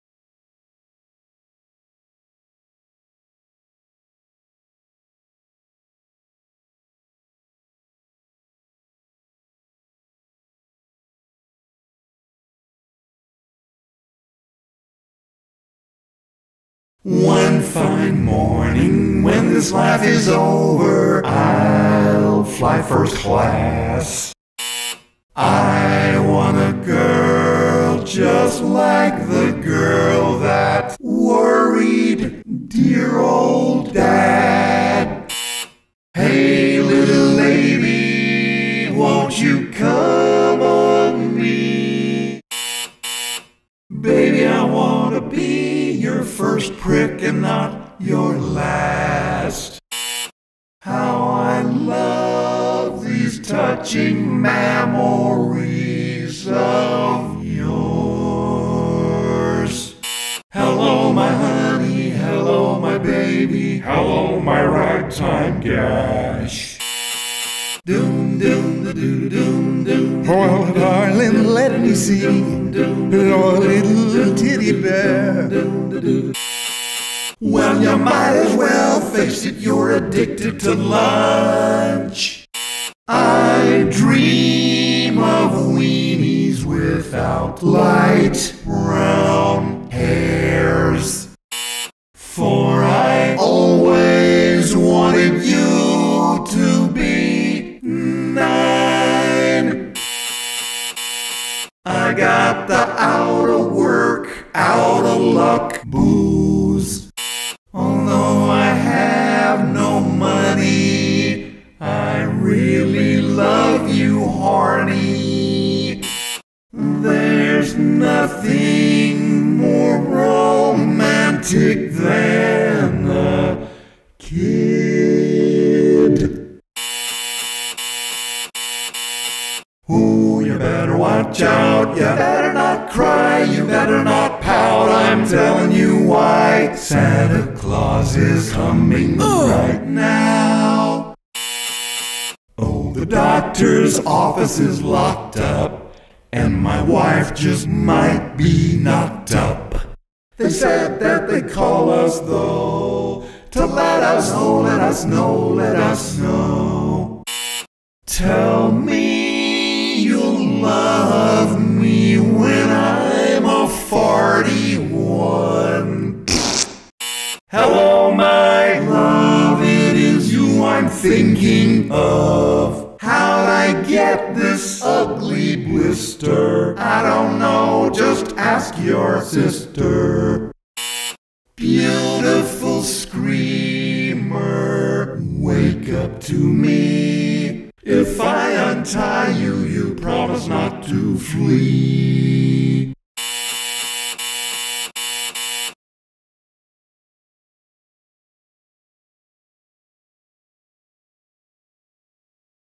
County Inmate Barbershop Quartet Bits
The most dangerous and ugly barbershop quartet in the world! They chop up their songs as badly as they do their victims!